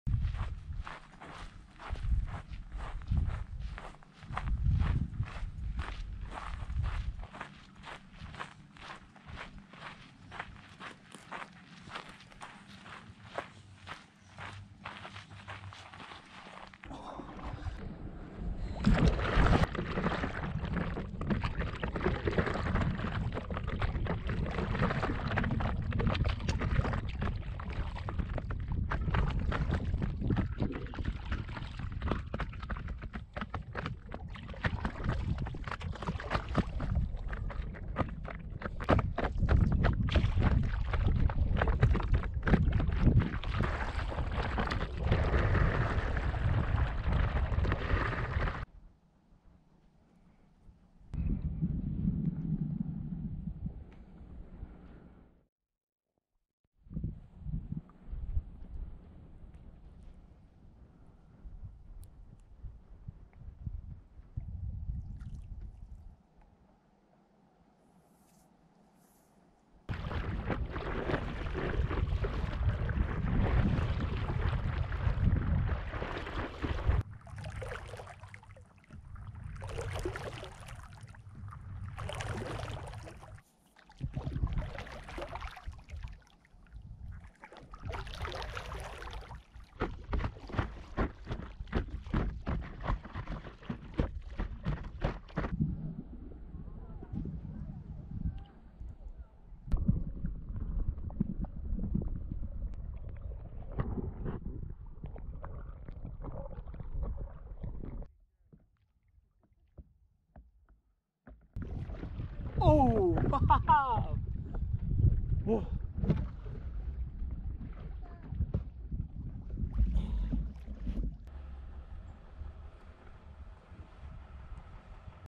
Mammoth Lakes - Lake Mary Paddleboarding
I visited mammoth lakes to paddleboard and chose Lake Mary. I think Lake Mary is one of the most beautiful lakes in Mammoth Lakes. It was amazing, beautiful and fantastic.

You will enjoy mountains view, basic with calm lakes. Very relaxing.

Hope you enjoy this and visit Mammoth Lakes.

Thanks